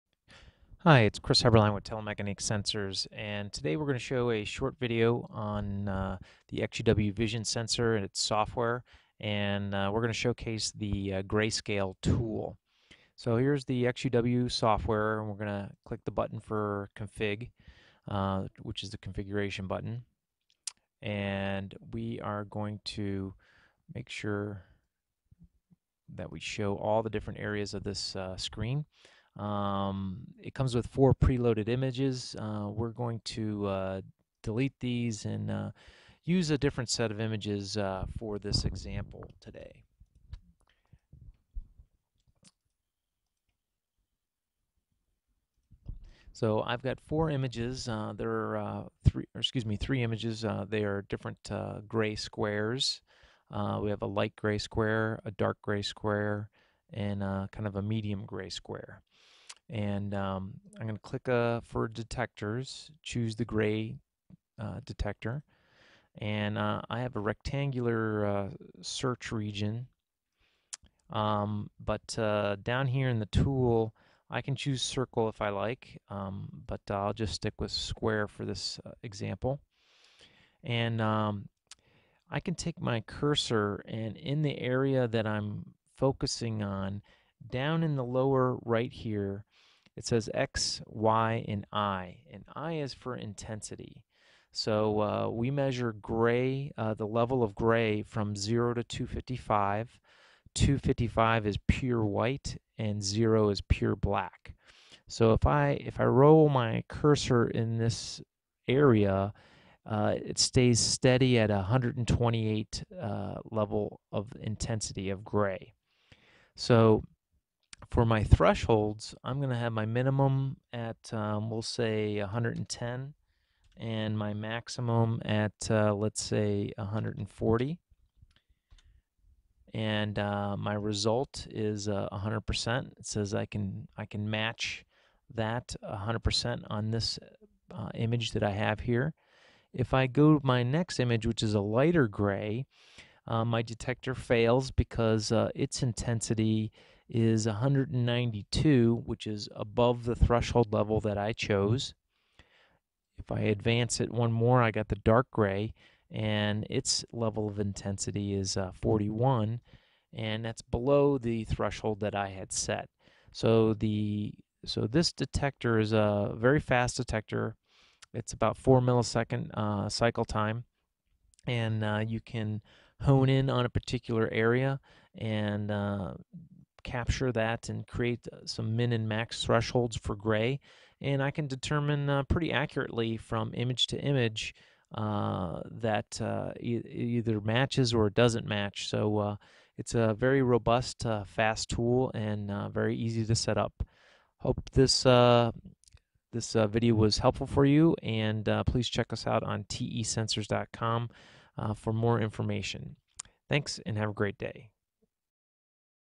Software tutorial - OsiSense XUW – Grey detector
This software tutorial explains you how to set the Grey detector up in the OsiSense XUW software.